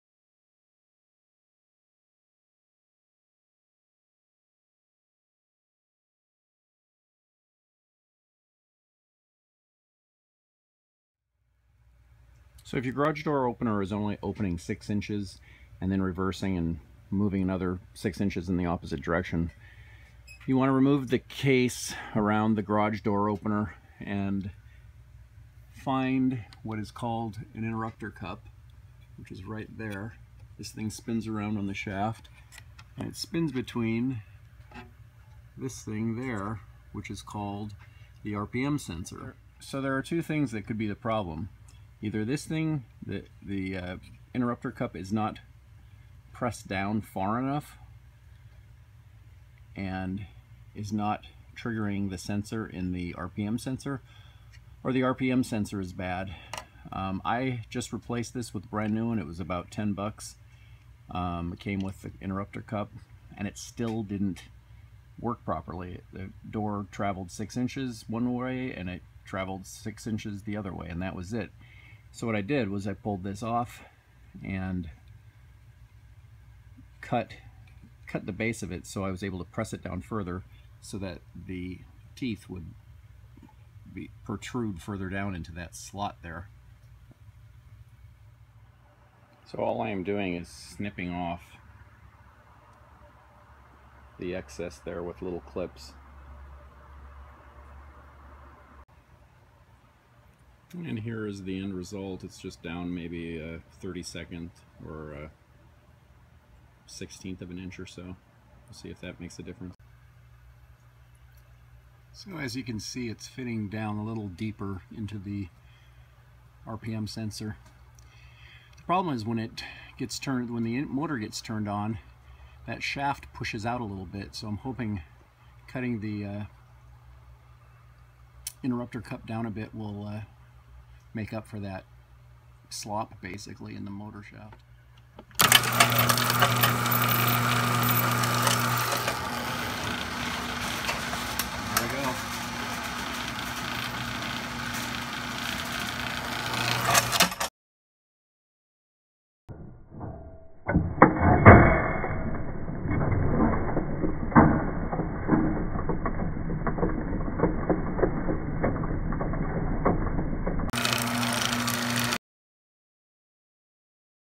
Garage Door Opener not working - Only Travels 6 Inches & Then Reverses & Stops
PROBLEM: Garage door only travels a little way and then reverses
Does your garage door only travel a little way, 6 inches or so, and then reverse?
It is most likely a problem with the RPM sensor and/or Interrupter cup.
1 - The sensor could be dirty - Solution - Clean it

2 - The Interrupter cup is not pressed on far enough to trigger the sensor in the RPM sensor - Press the cup down further

3 - The RPM sensor is bad - Solution - Replace the RPM Sensor

4 - If the motor shaft moves back and forth through the motor more than 1/16thof an inch during operation, it could lead to the door traveling only a short distance before reversing - Solution - Tighten the retaining collar so that the shaft does not move (most difficult solution).

Here are the Products I used:
Liftmaster 41C4398A RPM Sensor Assembly

Garage Door Opener Gear Kit 41A2817 for Chamberlain Craftsman LiftMaster Sears